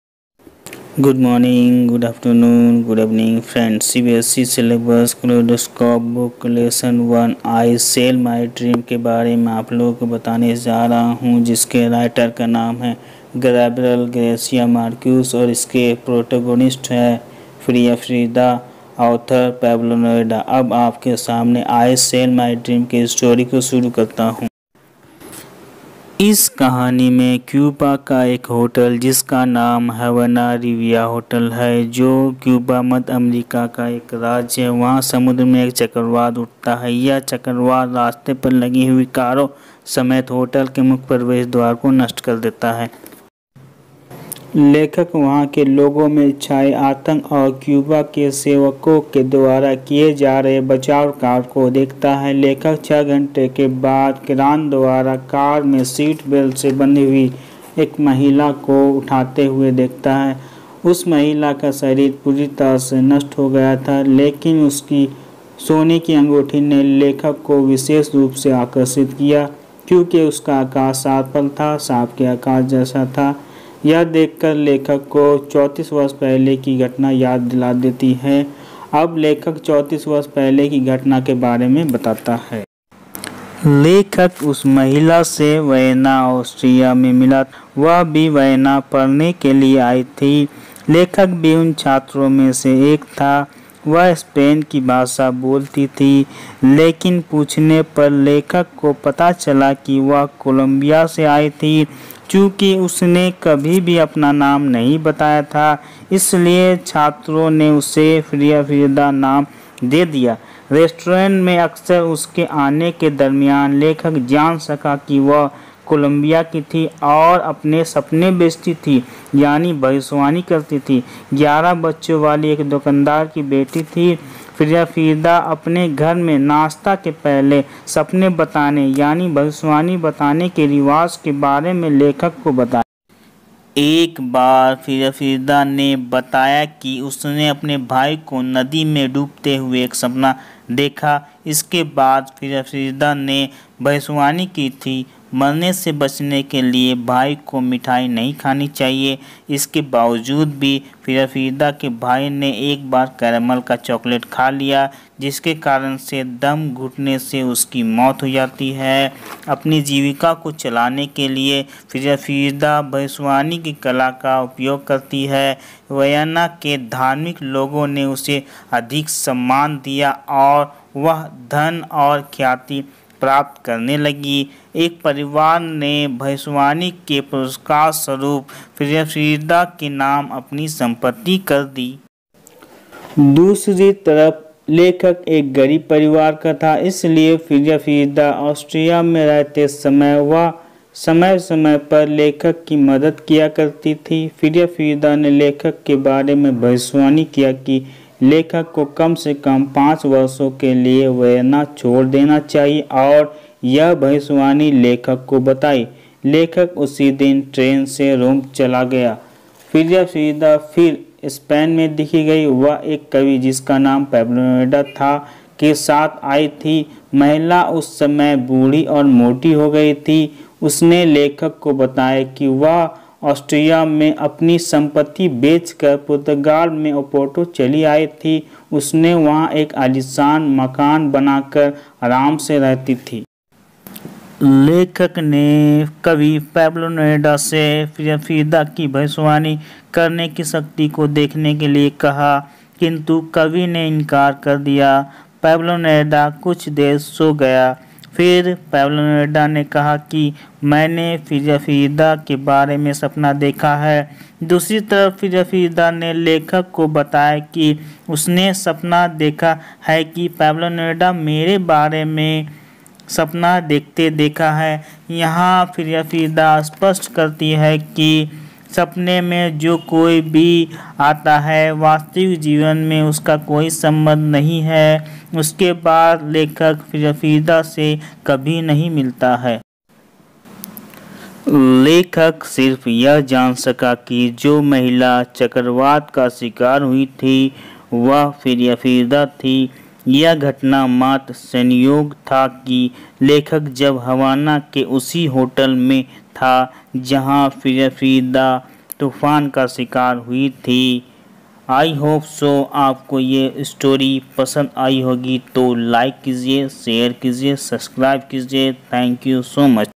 Lesson - 1- I sell my dream , kaleidoscope book
CBSE
Class - 12
kaleidoscope book.
Lesson -1- I sell my dream
Writer - Gabriel Garcia Marquz
this is story about a woman named Frau Frieda.
The place in this story is Havana Riviera hotel in Cuba . Where the woman had succumbed to the sea storm in her car.